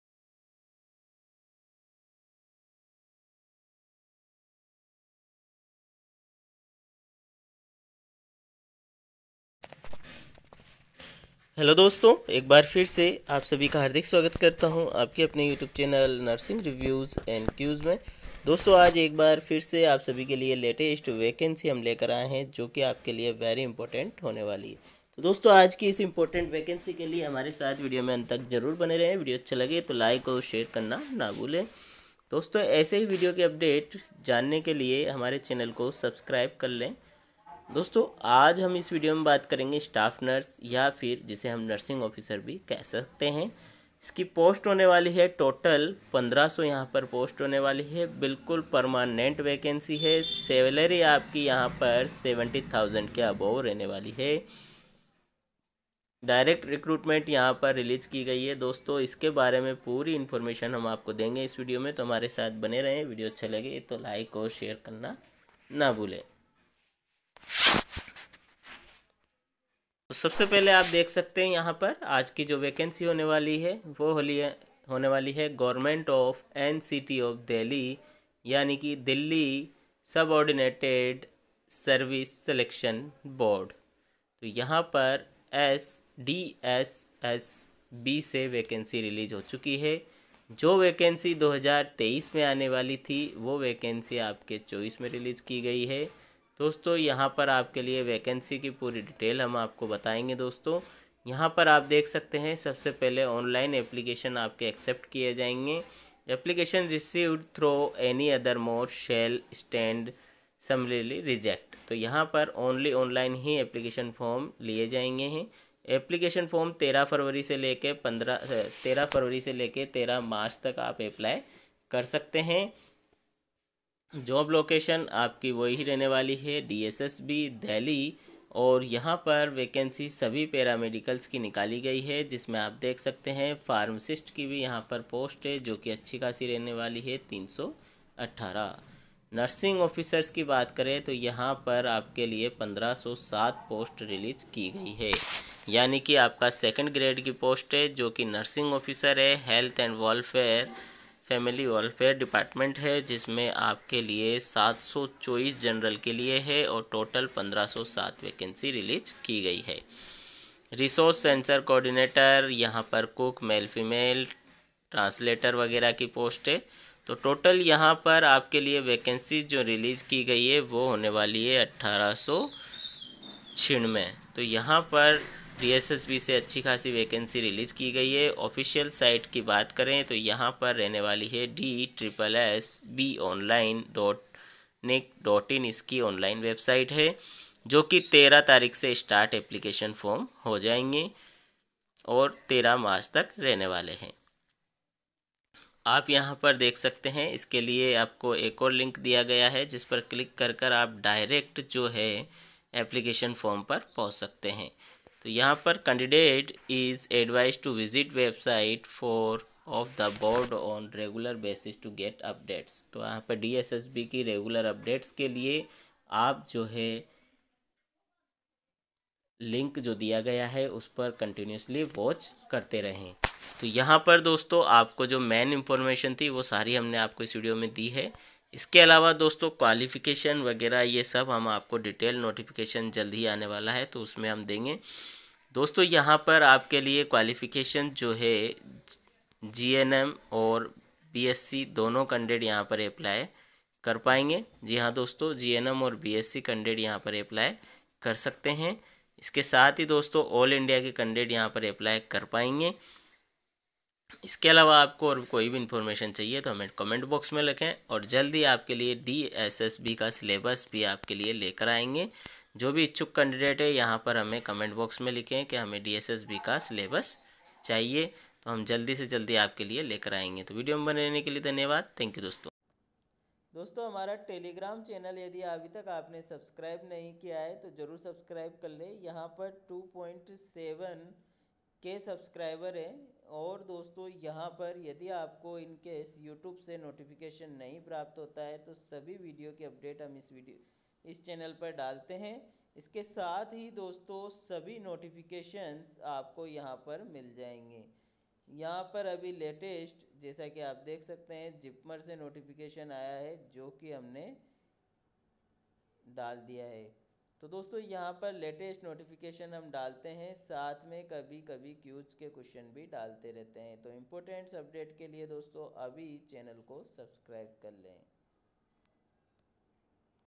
Delhi से आप के लिये बिल्कुल परमानेंट भर्ती, Nursing Officer Vacancy 2024,All INDIA Apply, Salary-70k
👉Gynecology and Obstetrics Golden Key Points:~

Any Queries Write in comment Box.